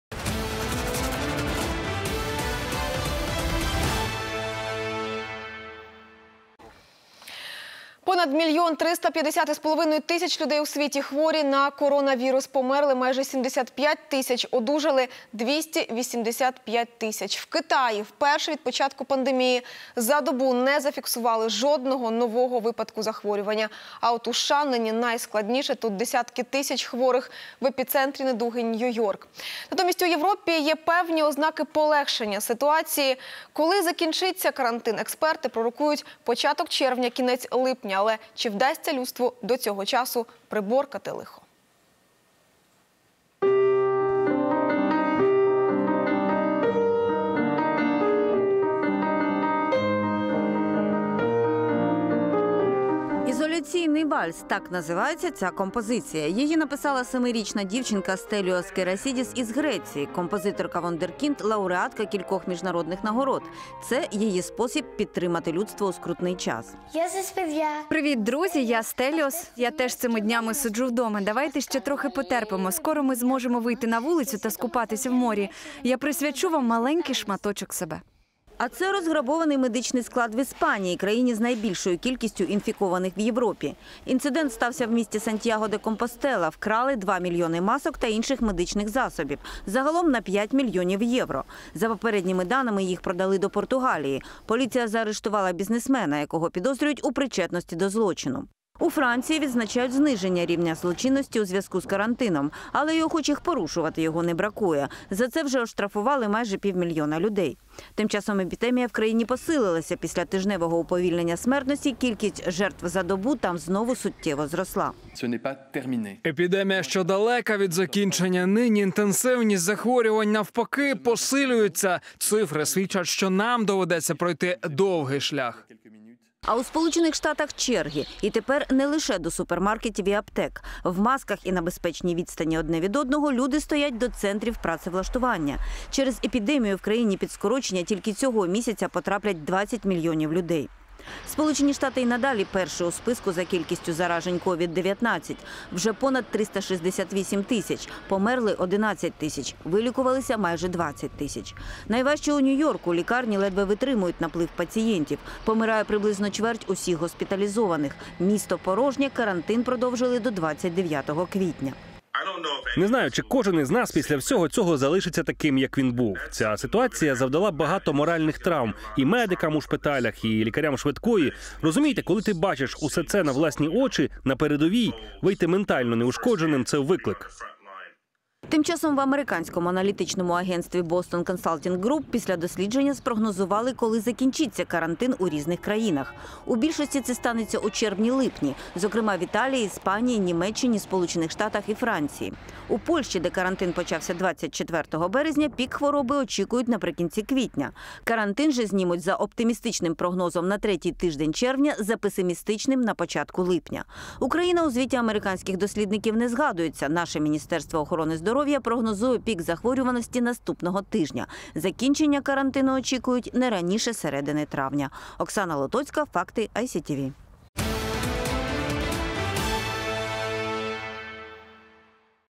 Коронавирус в мире: переполненные больницы в США и мародерство в Испании
Более 1,3 млн человек в мире заразились коронавирусом, умерло - 74 816, выздоровели почти 285 тыс. Самае сложная ситуация сейчас в США, а в Европе появились определенные признаки облегчения ситуации. Глобальные катастрофы всегда открывают в людях различные качества. Для кого-то это - возможность помочь и поддержать окружающих, для других - источник наживы.

В Испании, где наибольшее количество инфицированных в Европе, разграбили медицинский склад. Инцидент произошел городе Сантьяго-де-Компостелла. Украли 2 млн масок и других медицинских средств на 5 млн евро. По предварительным данным, их продали в Португалию. Полиция арестовала бизнесмена, которого подозревают в причастности к преступлению. Подробнее в сюжете Оксаны Лотоцкой.